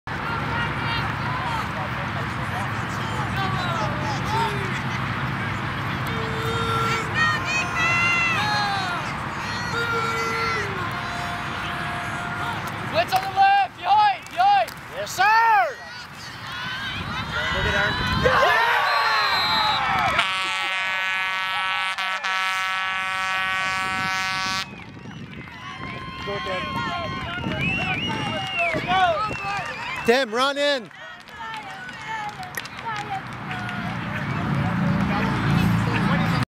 2011 Western Patriotes Peewee AAA Football - Can't be covered?
Pinpoint pass from #12 Desmond Sanon to #44 Aaron Campbell. Bringing it down in double coverage!